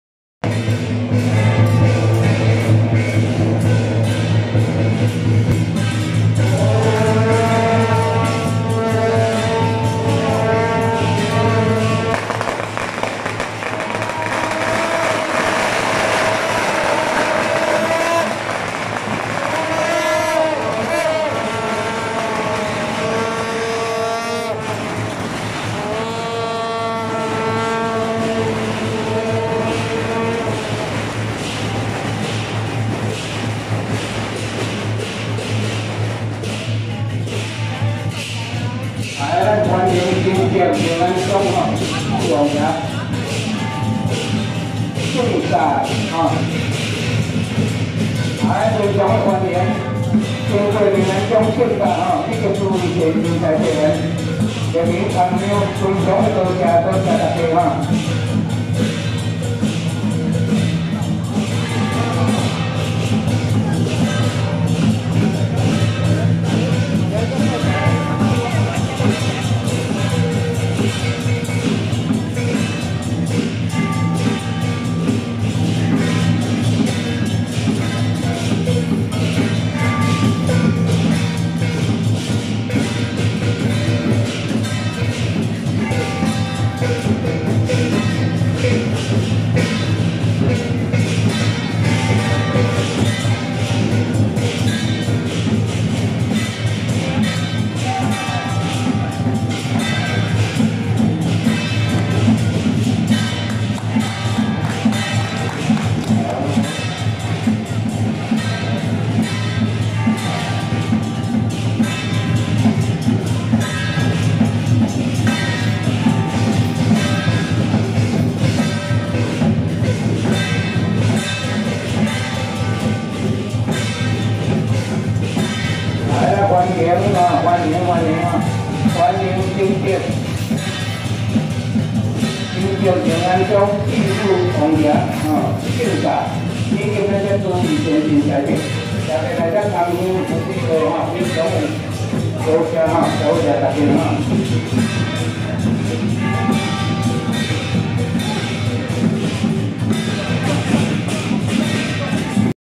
【新竹哨角＆新竹三塊厝靈安宮-小轎(齊天大聖 聖駕)】~110新竹三塊厝靈安宮邱吳溫周四府王爺往淡水和衷宮參香
新竹哨角＆新竹三塊厝靈安宮-小轎(齊天大聖 聖駕)